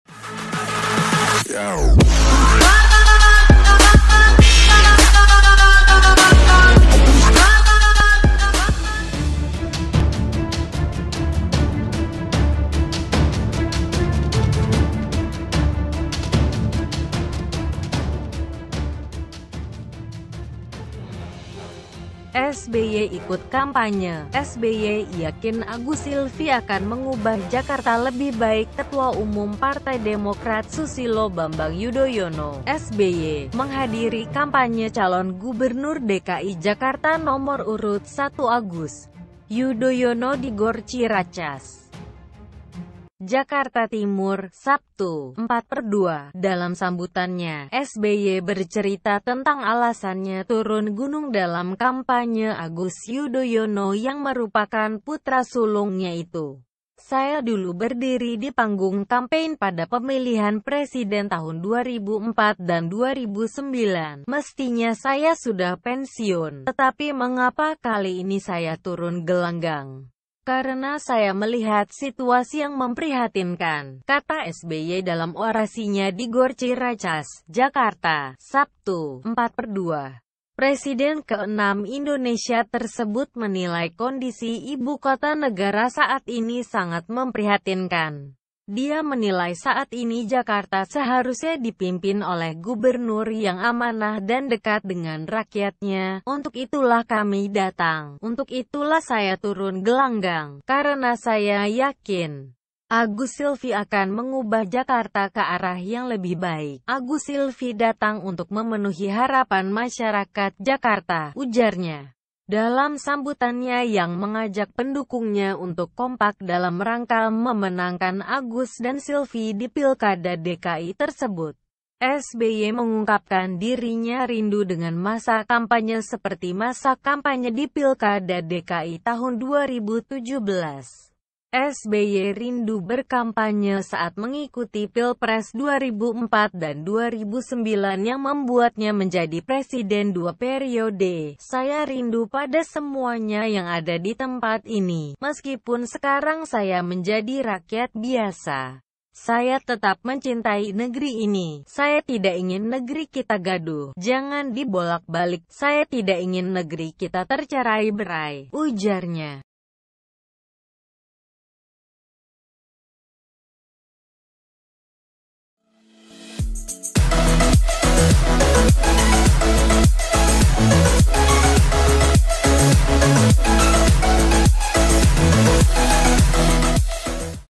SBY Ikut Kampanye | SBY Yakin Agus-Sylvi Akan Mengubah Jakarta Lebih Baik
Ketua Umum Partai Demokrat Susilo Bambang Yudhoyono (SBY) menghadiri kampanye calon Gubernur DKI Jakarta nomor urut 1 Agus Yudhoyono di GOR Ciracas, Jakarta Timur, Sabtu (4/2). Dalam sambutannya, SBY bercerita tentang alasannya turun gunung dalam kampanye Agus Yudhoyono yang merupakan putra sulungnya itu.

"Saya dulu berdiri di panggung kampanye pada pemilihan presiden tahun 2004 dan 2009. Mestinya saya sudah pensiun. Tetapi mengapa kali ini saya turun gelanggang, karena saya melihat situasi yang memprihatinkan," kata SBY dalam orasinya di GOR Ciracas, Jakarta, Sabtu (4/2).

Sumber Berita : Merdeka.com